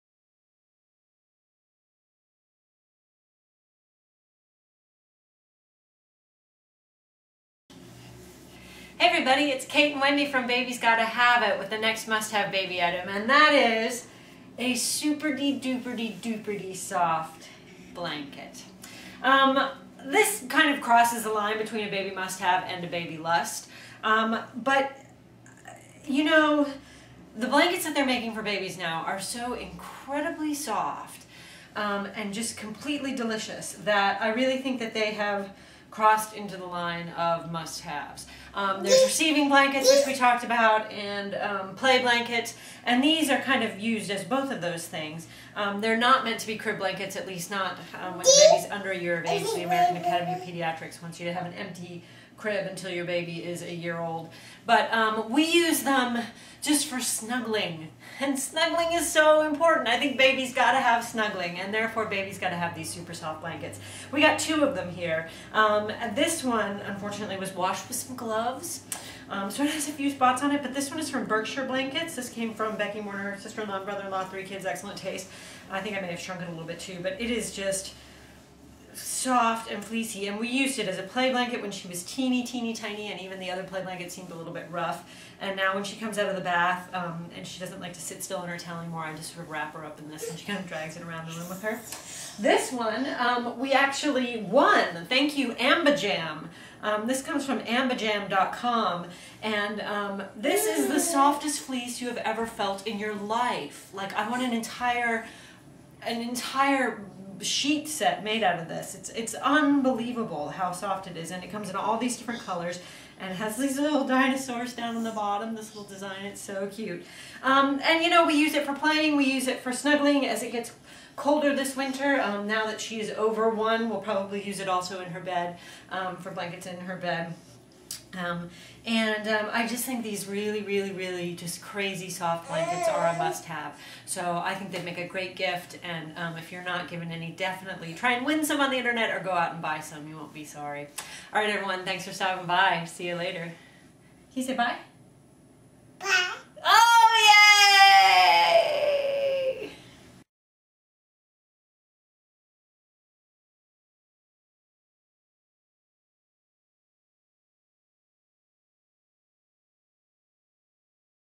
Soft Blankets for Kids - Because Snuggling is Essential
Babies Gotta Have It dishes on super soft blankets for babies.